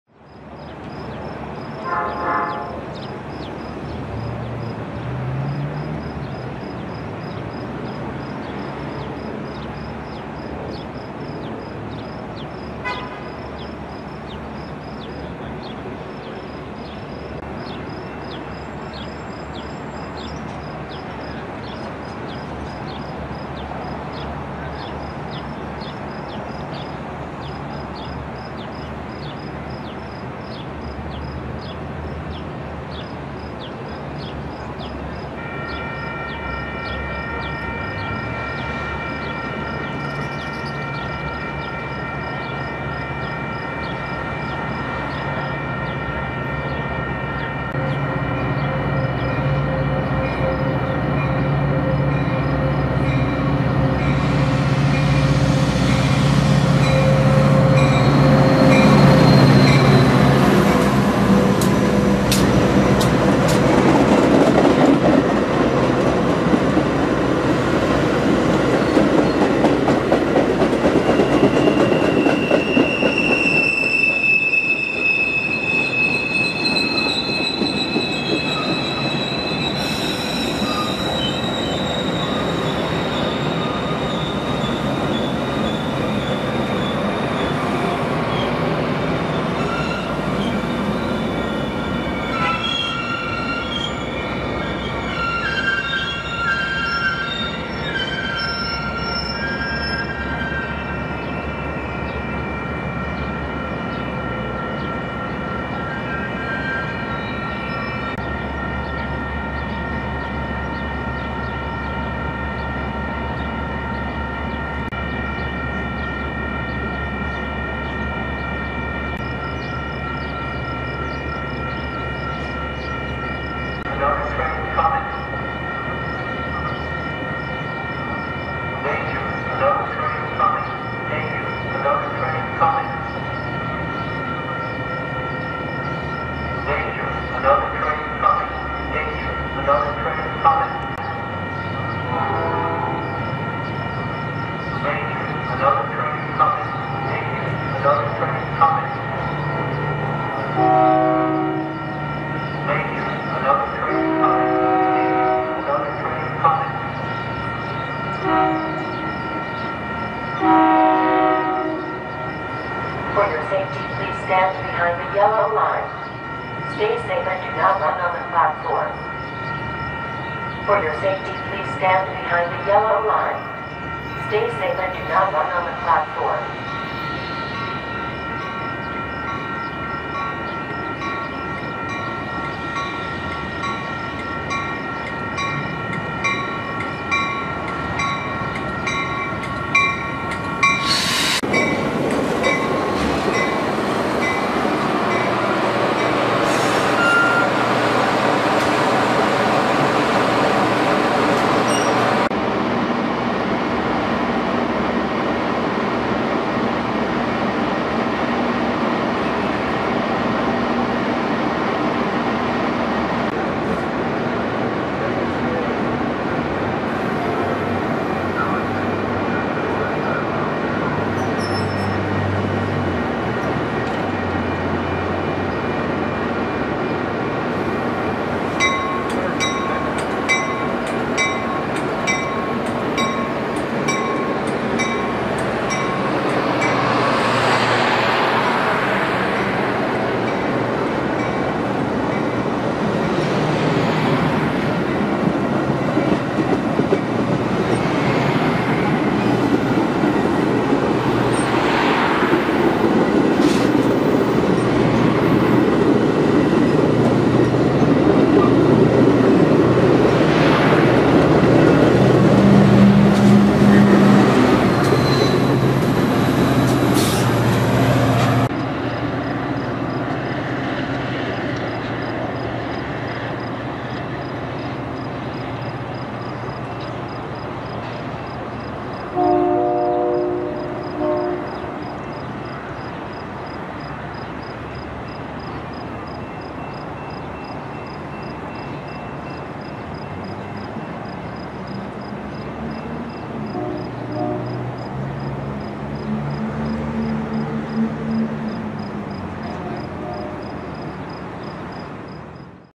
Metra Meet With Fares Amidst The Rails At York Street
A very nice look at two Metra commuter trains as they enter and leave the busy Elmhurst Station which sits on the UPRR Chicago West Line. The Outbound train was on the center train due to track work just east of the station. Both trains were there at the same time and then the Outbound left and shortly thereafter, the Inbound started east.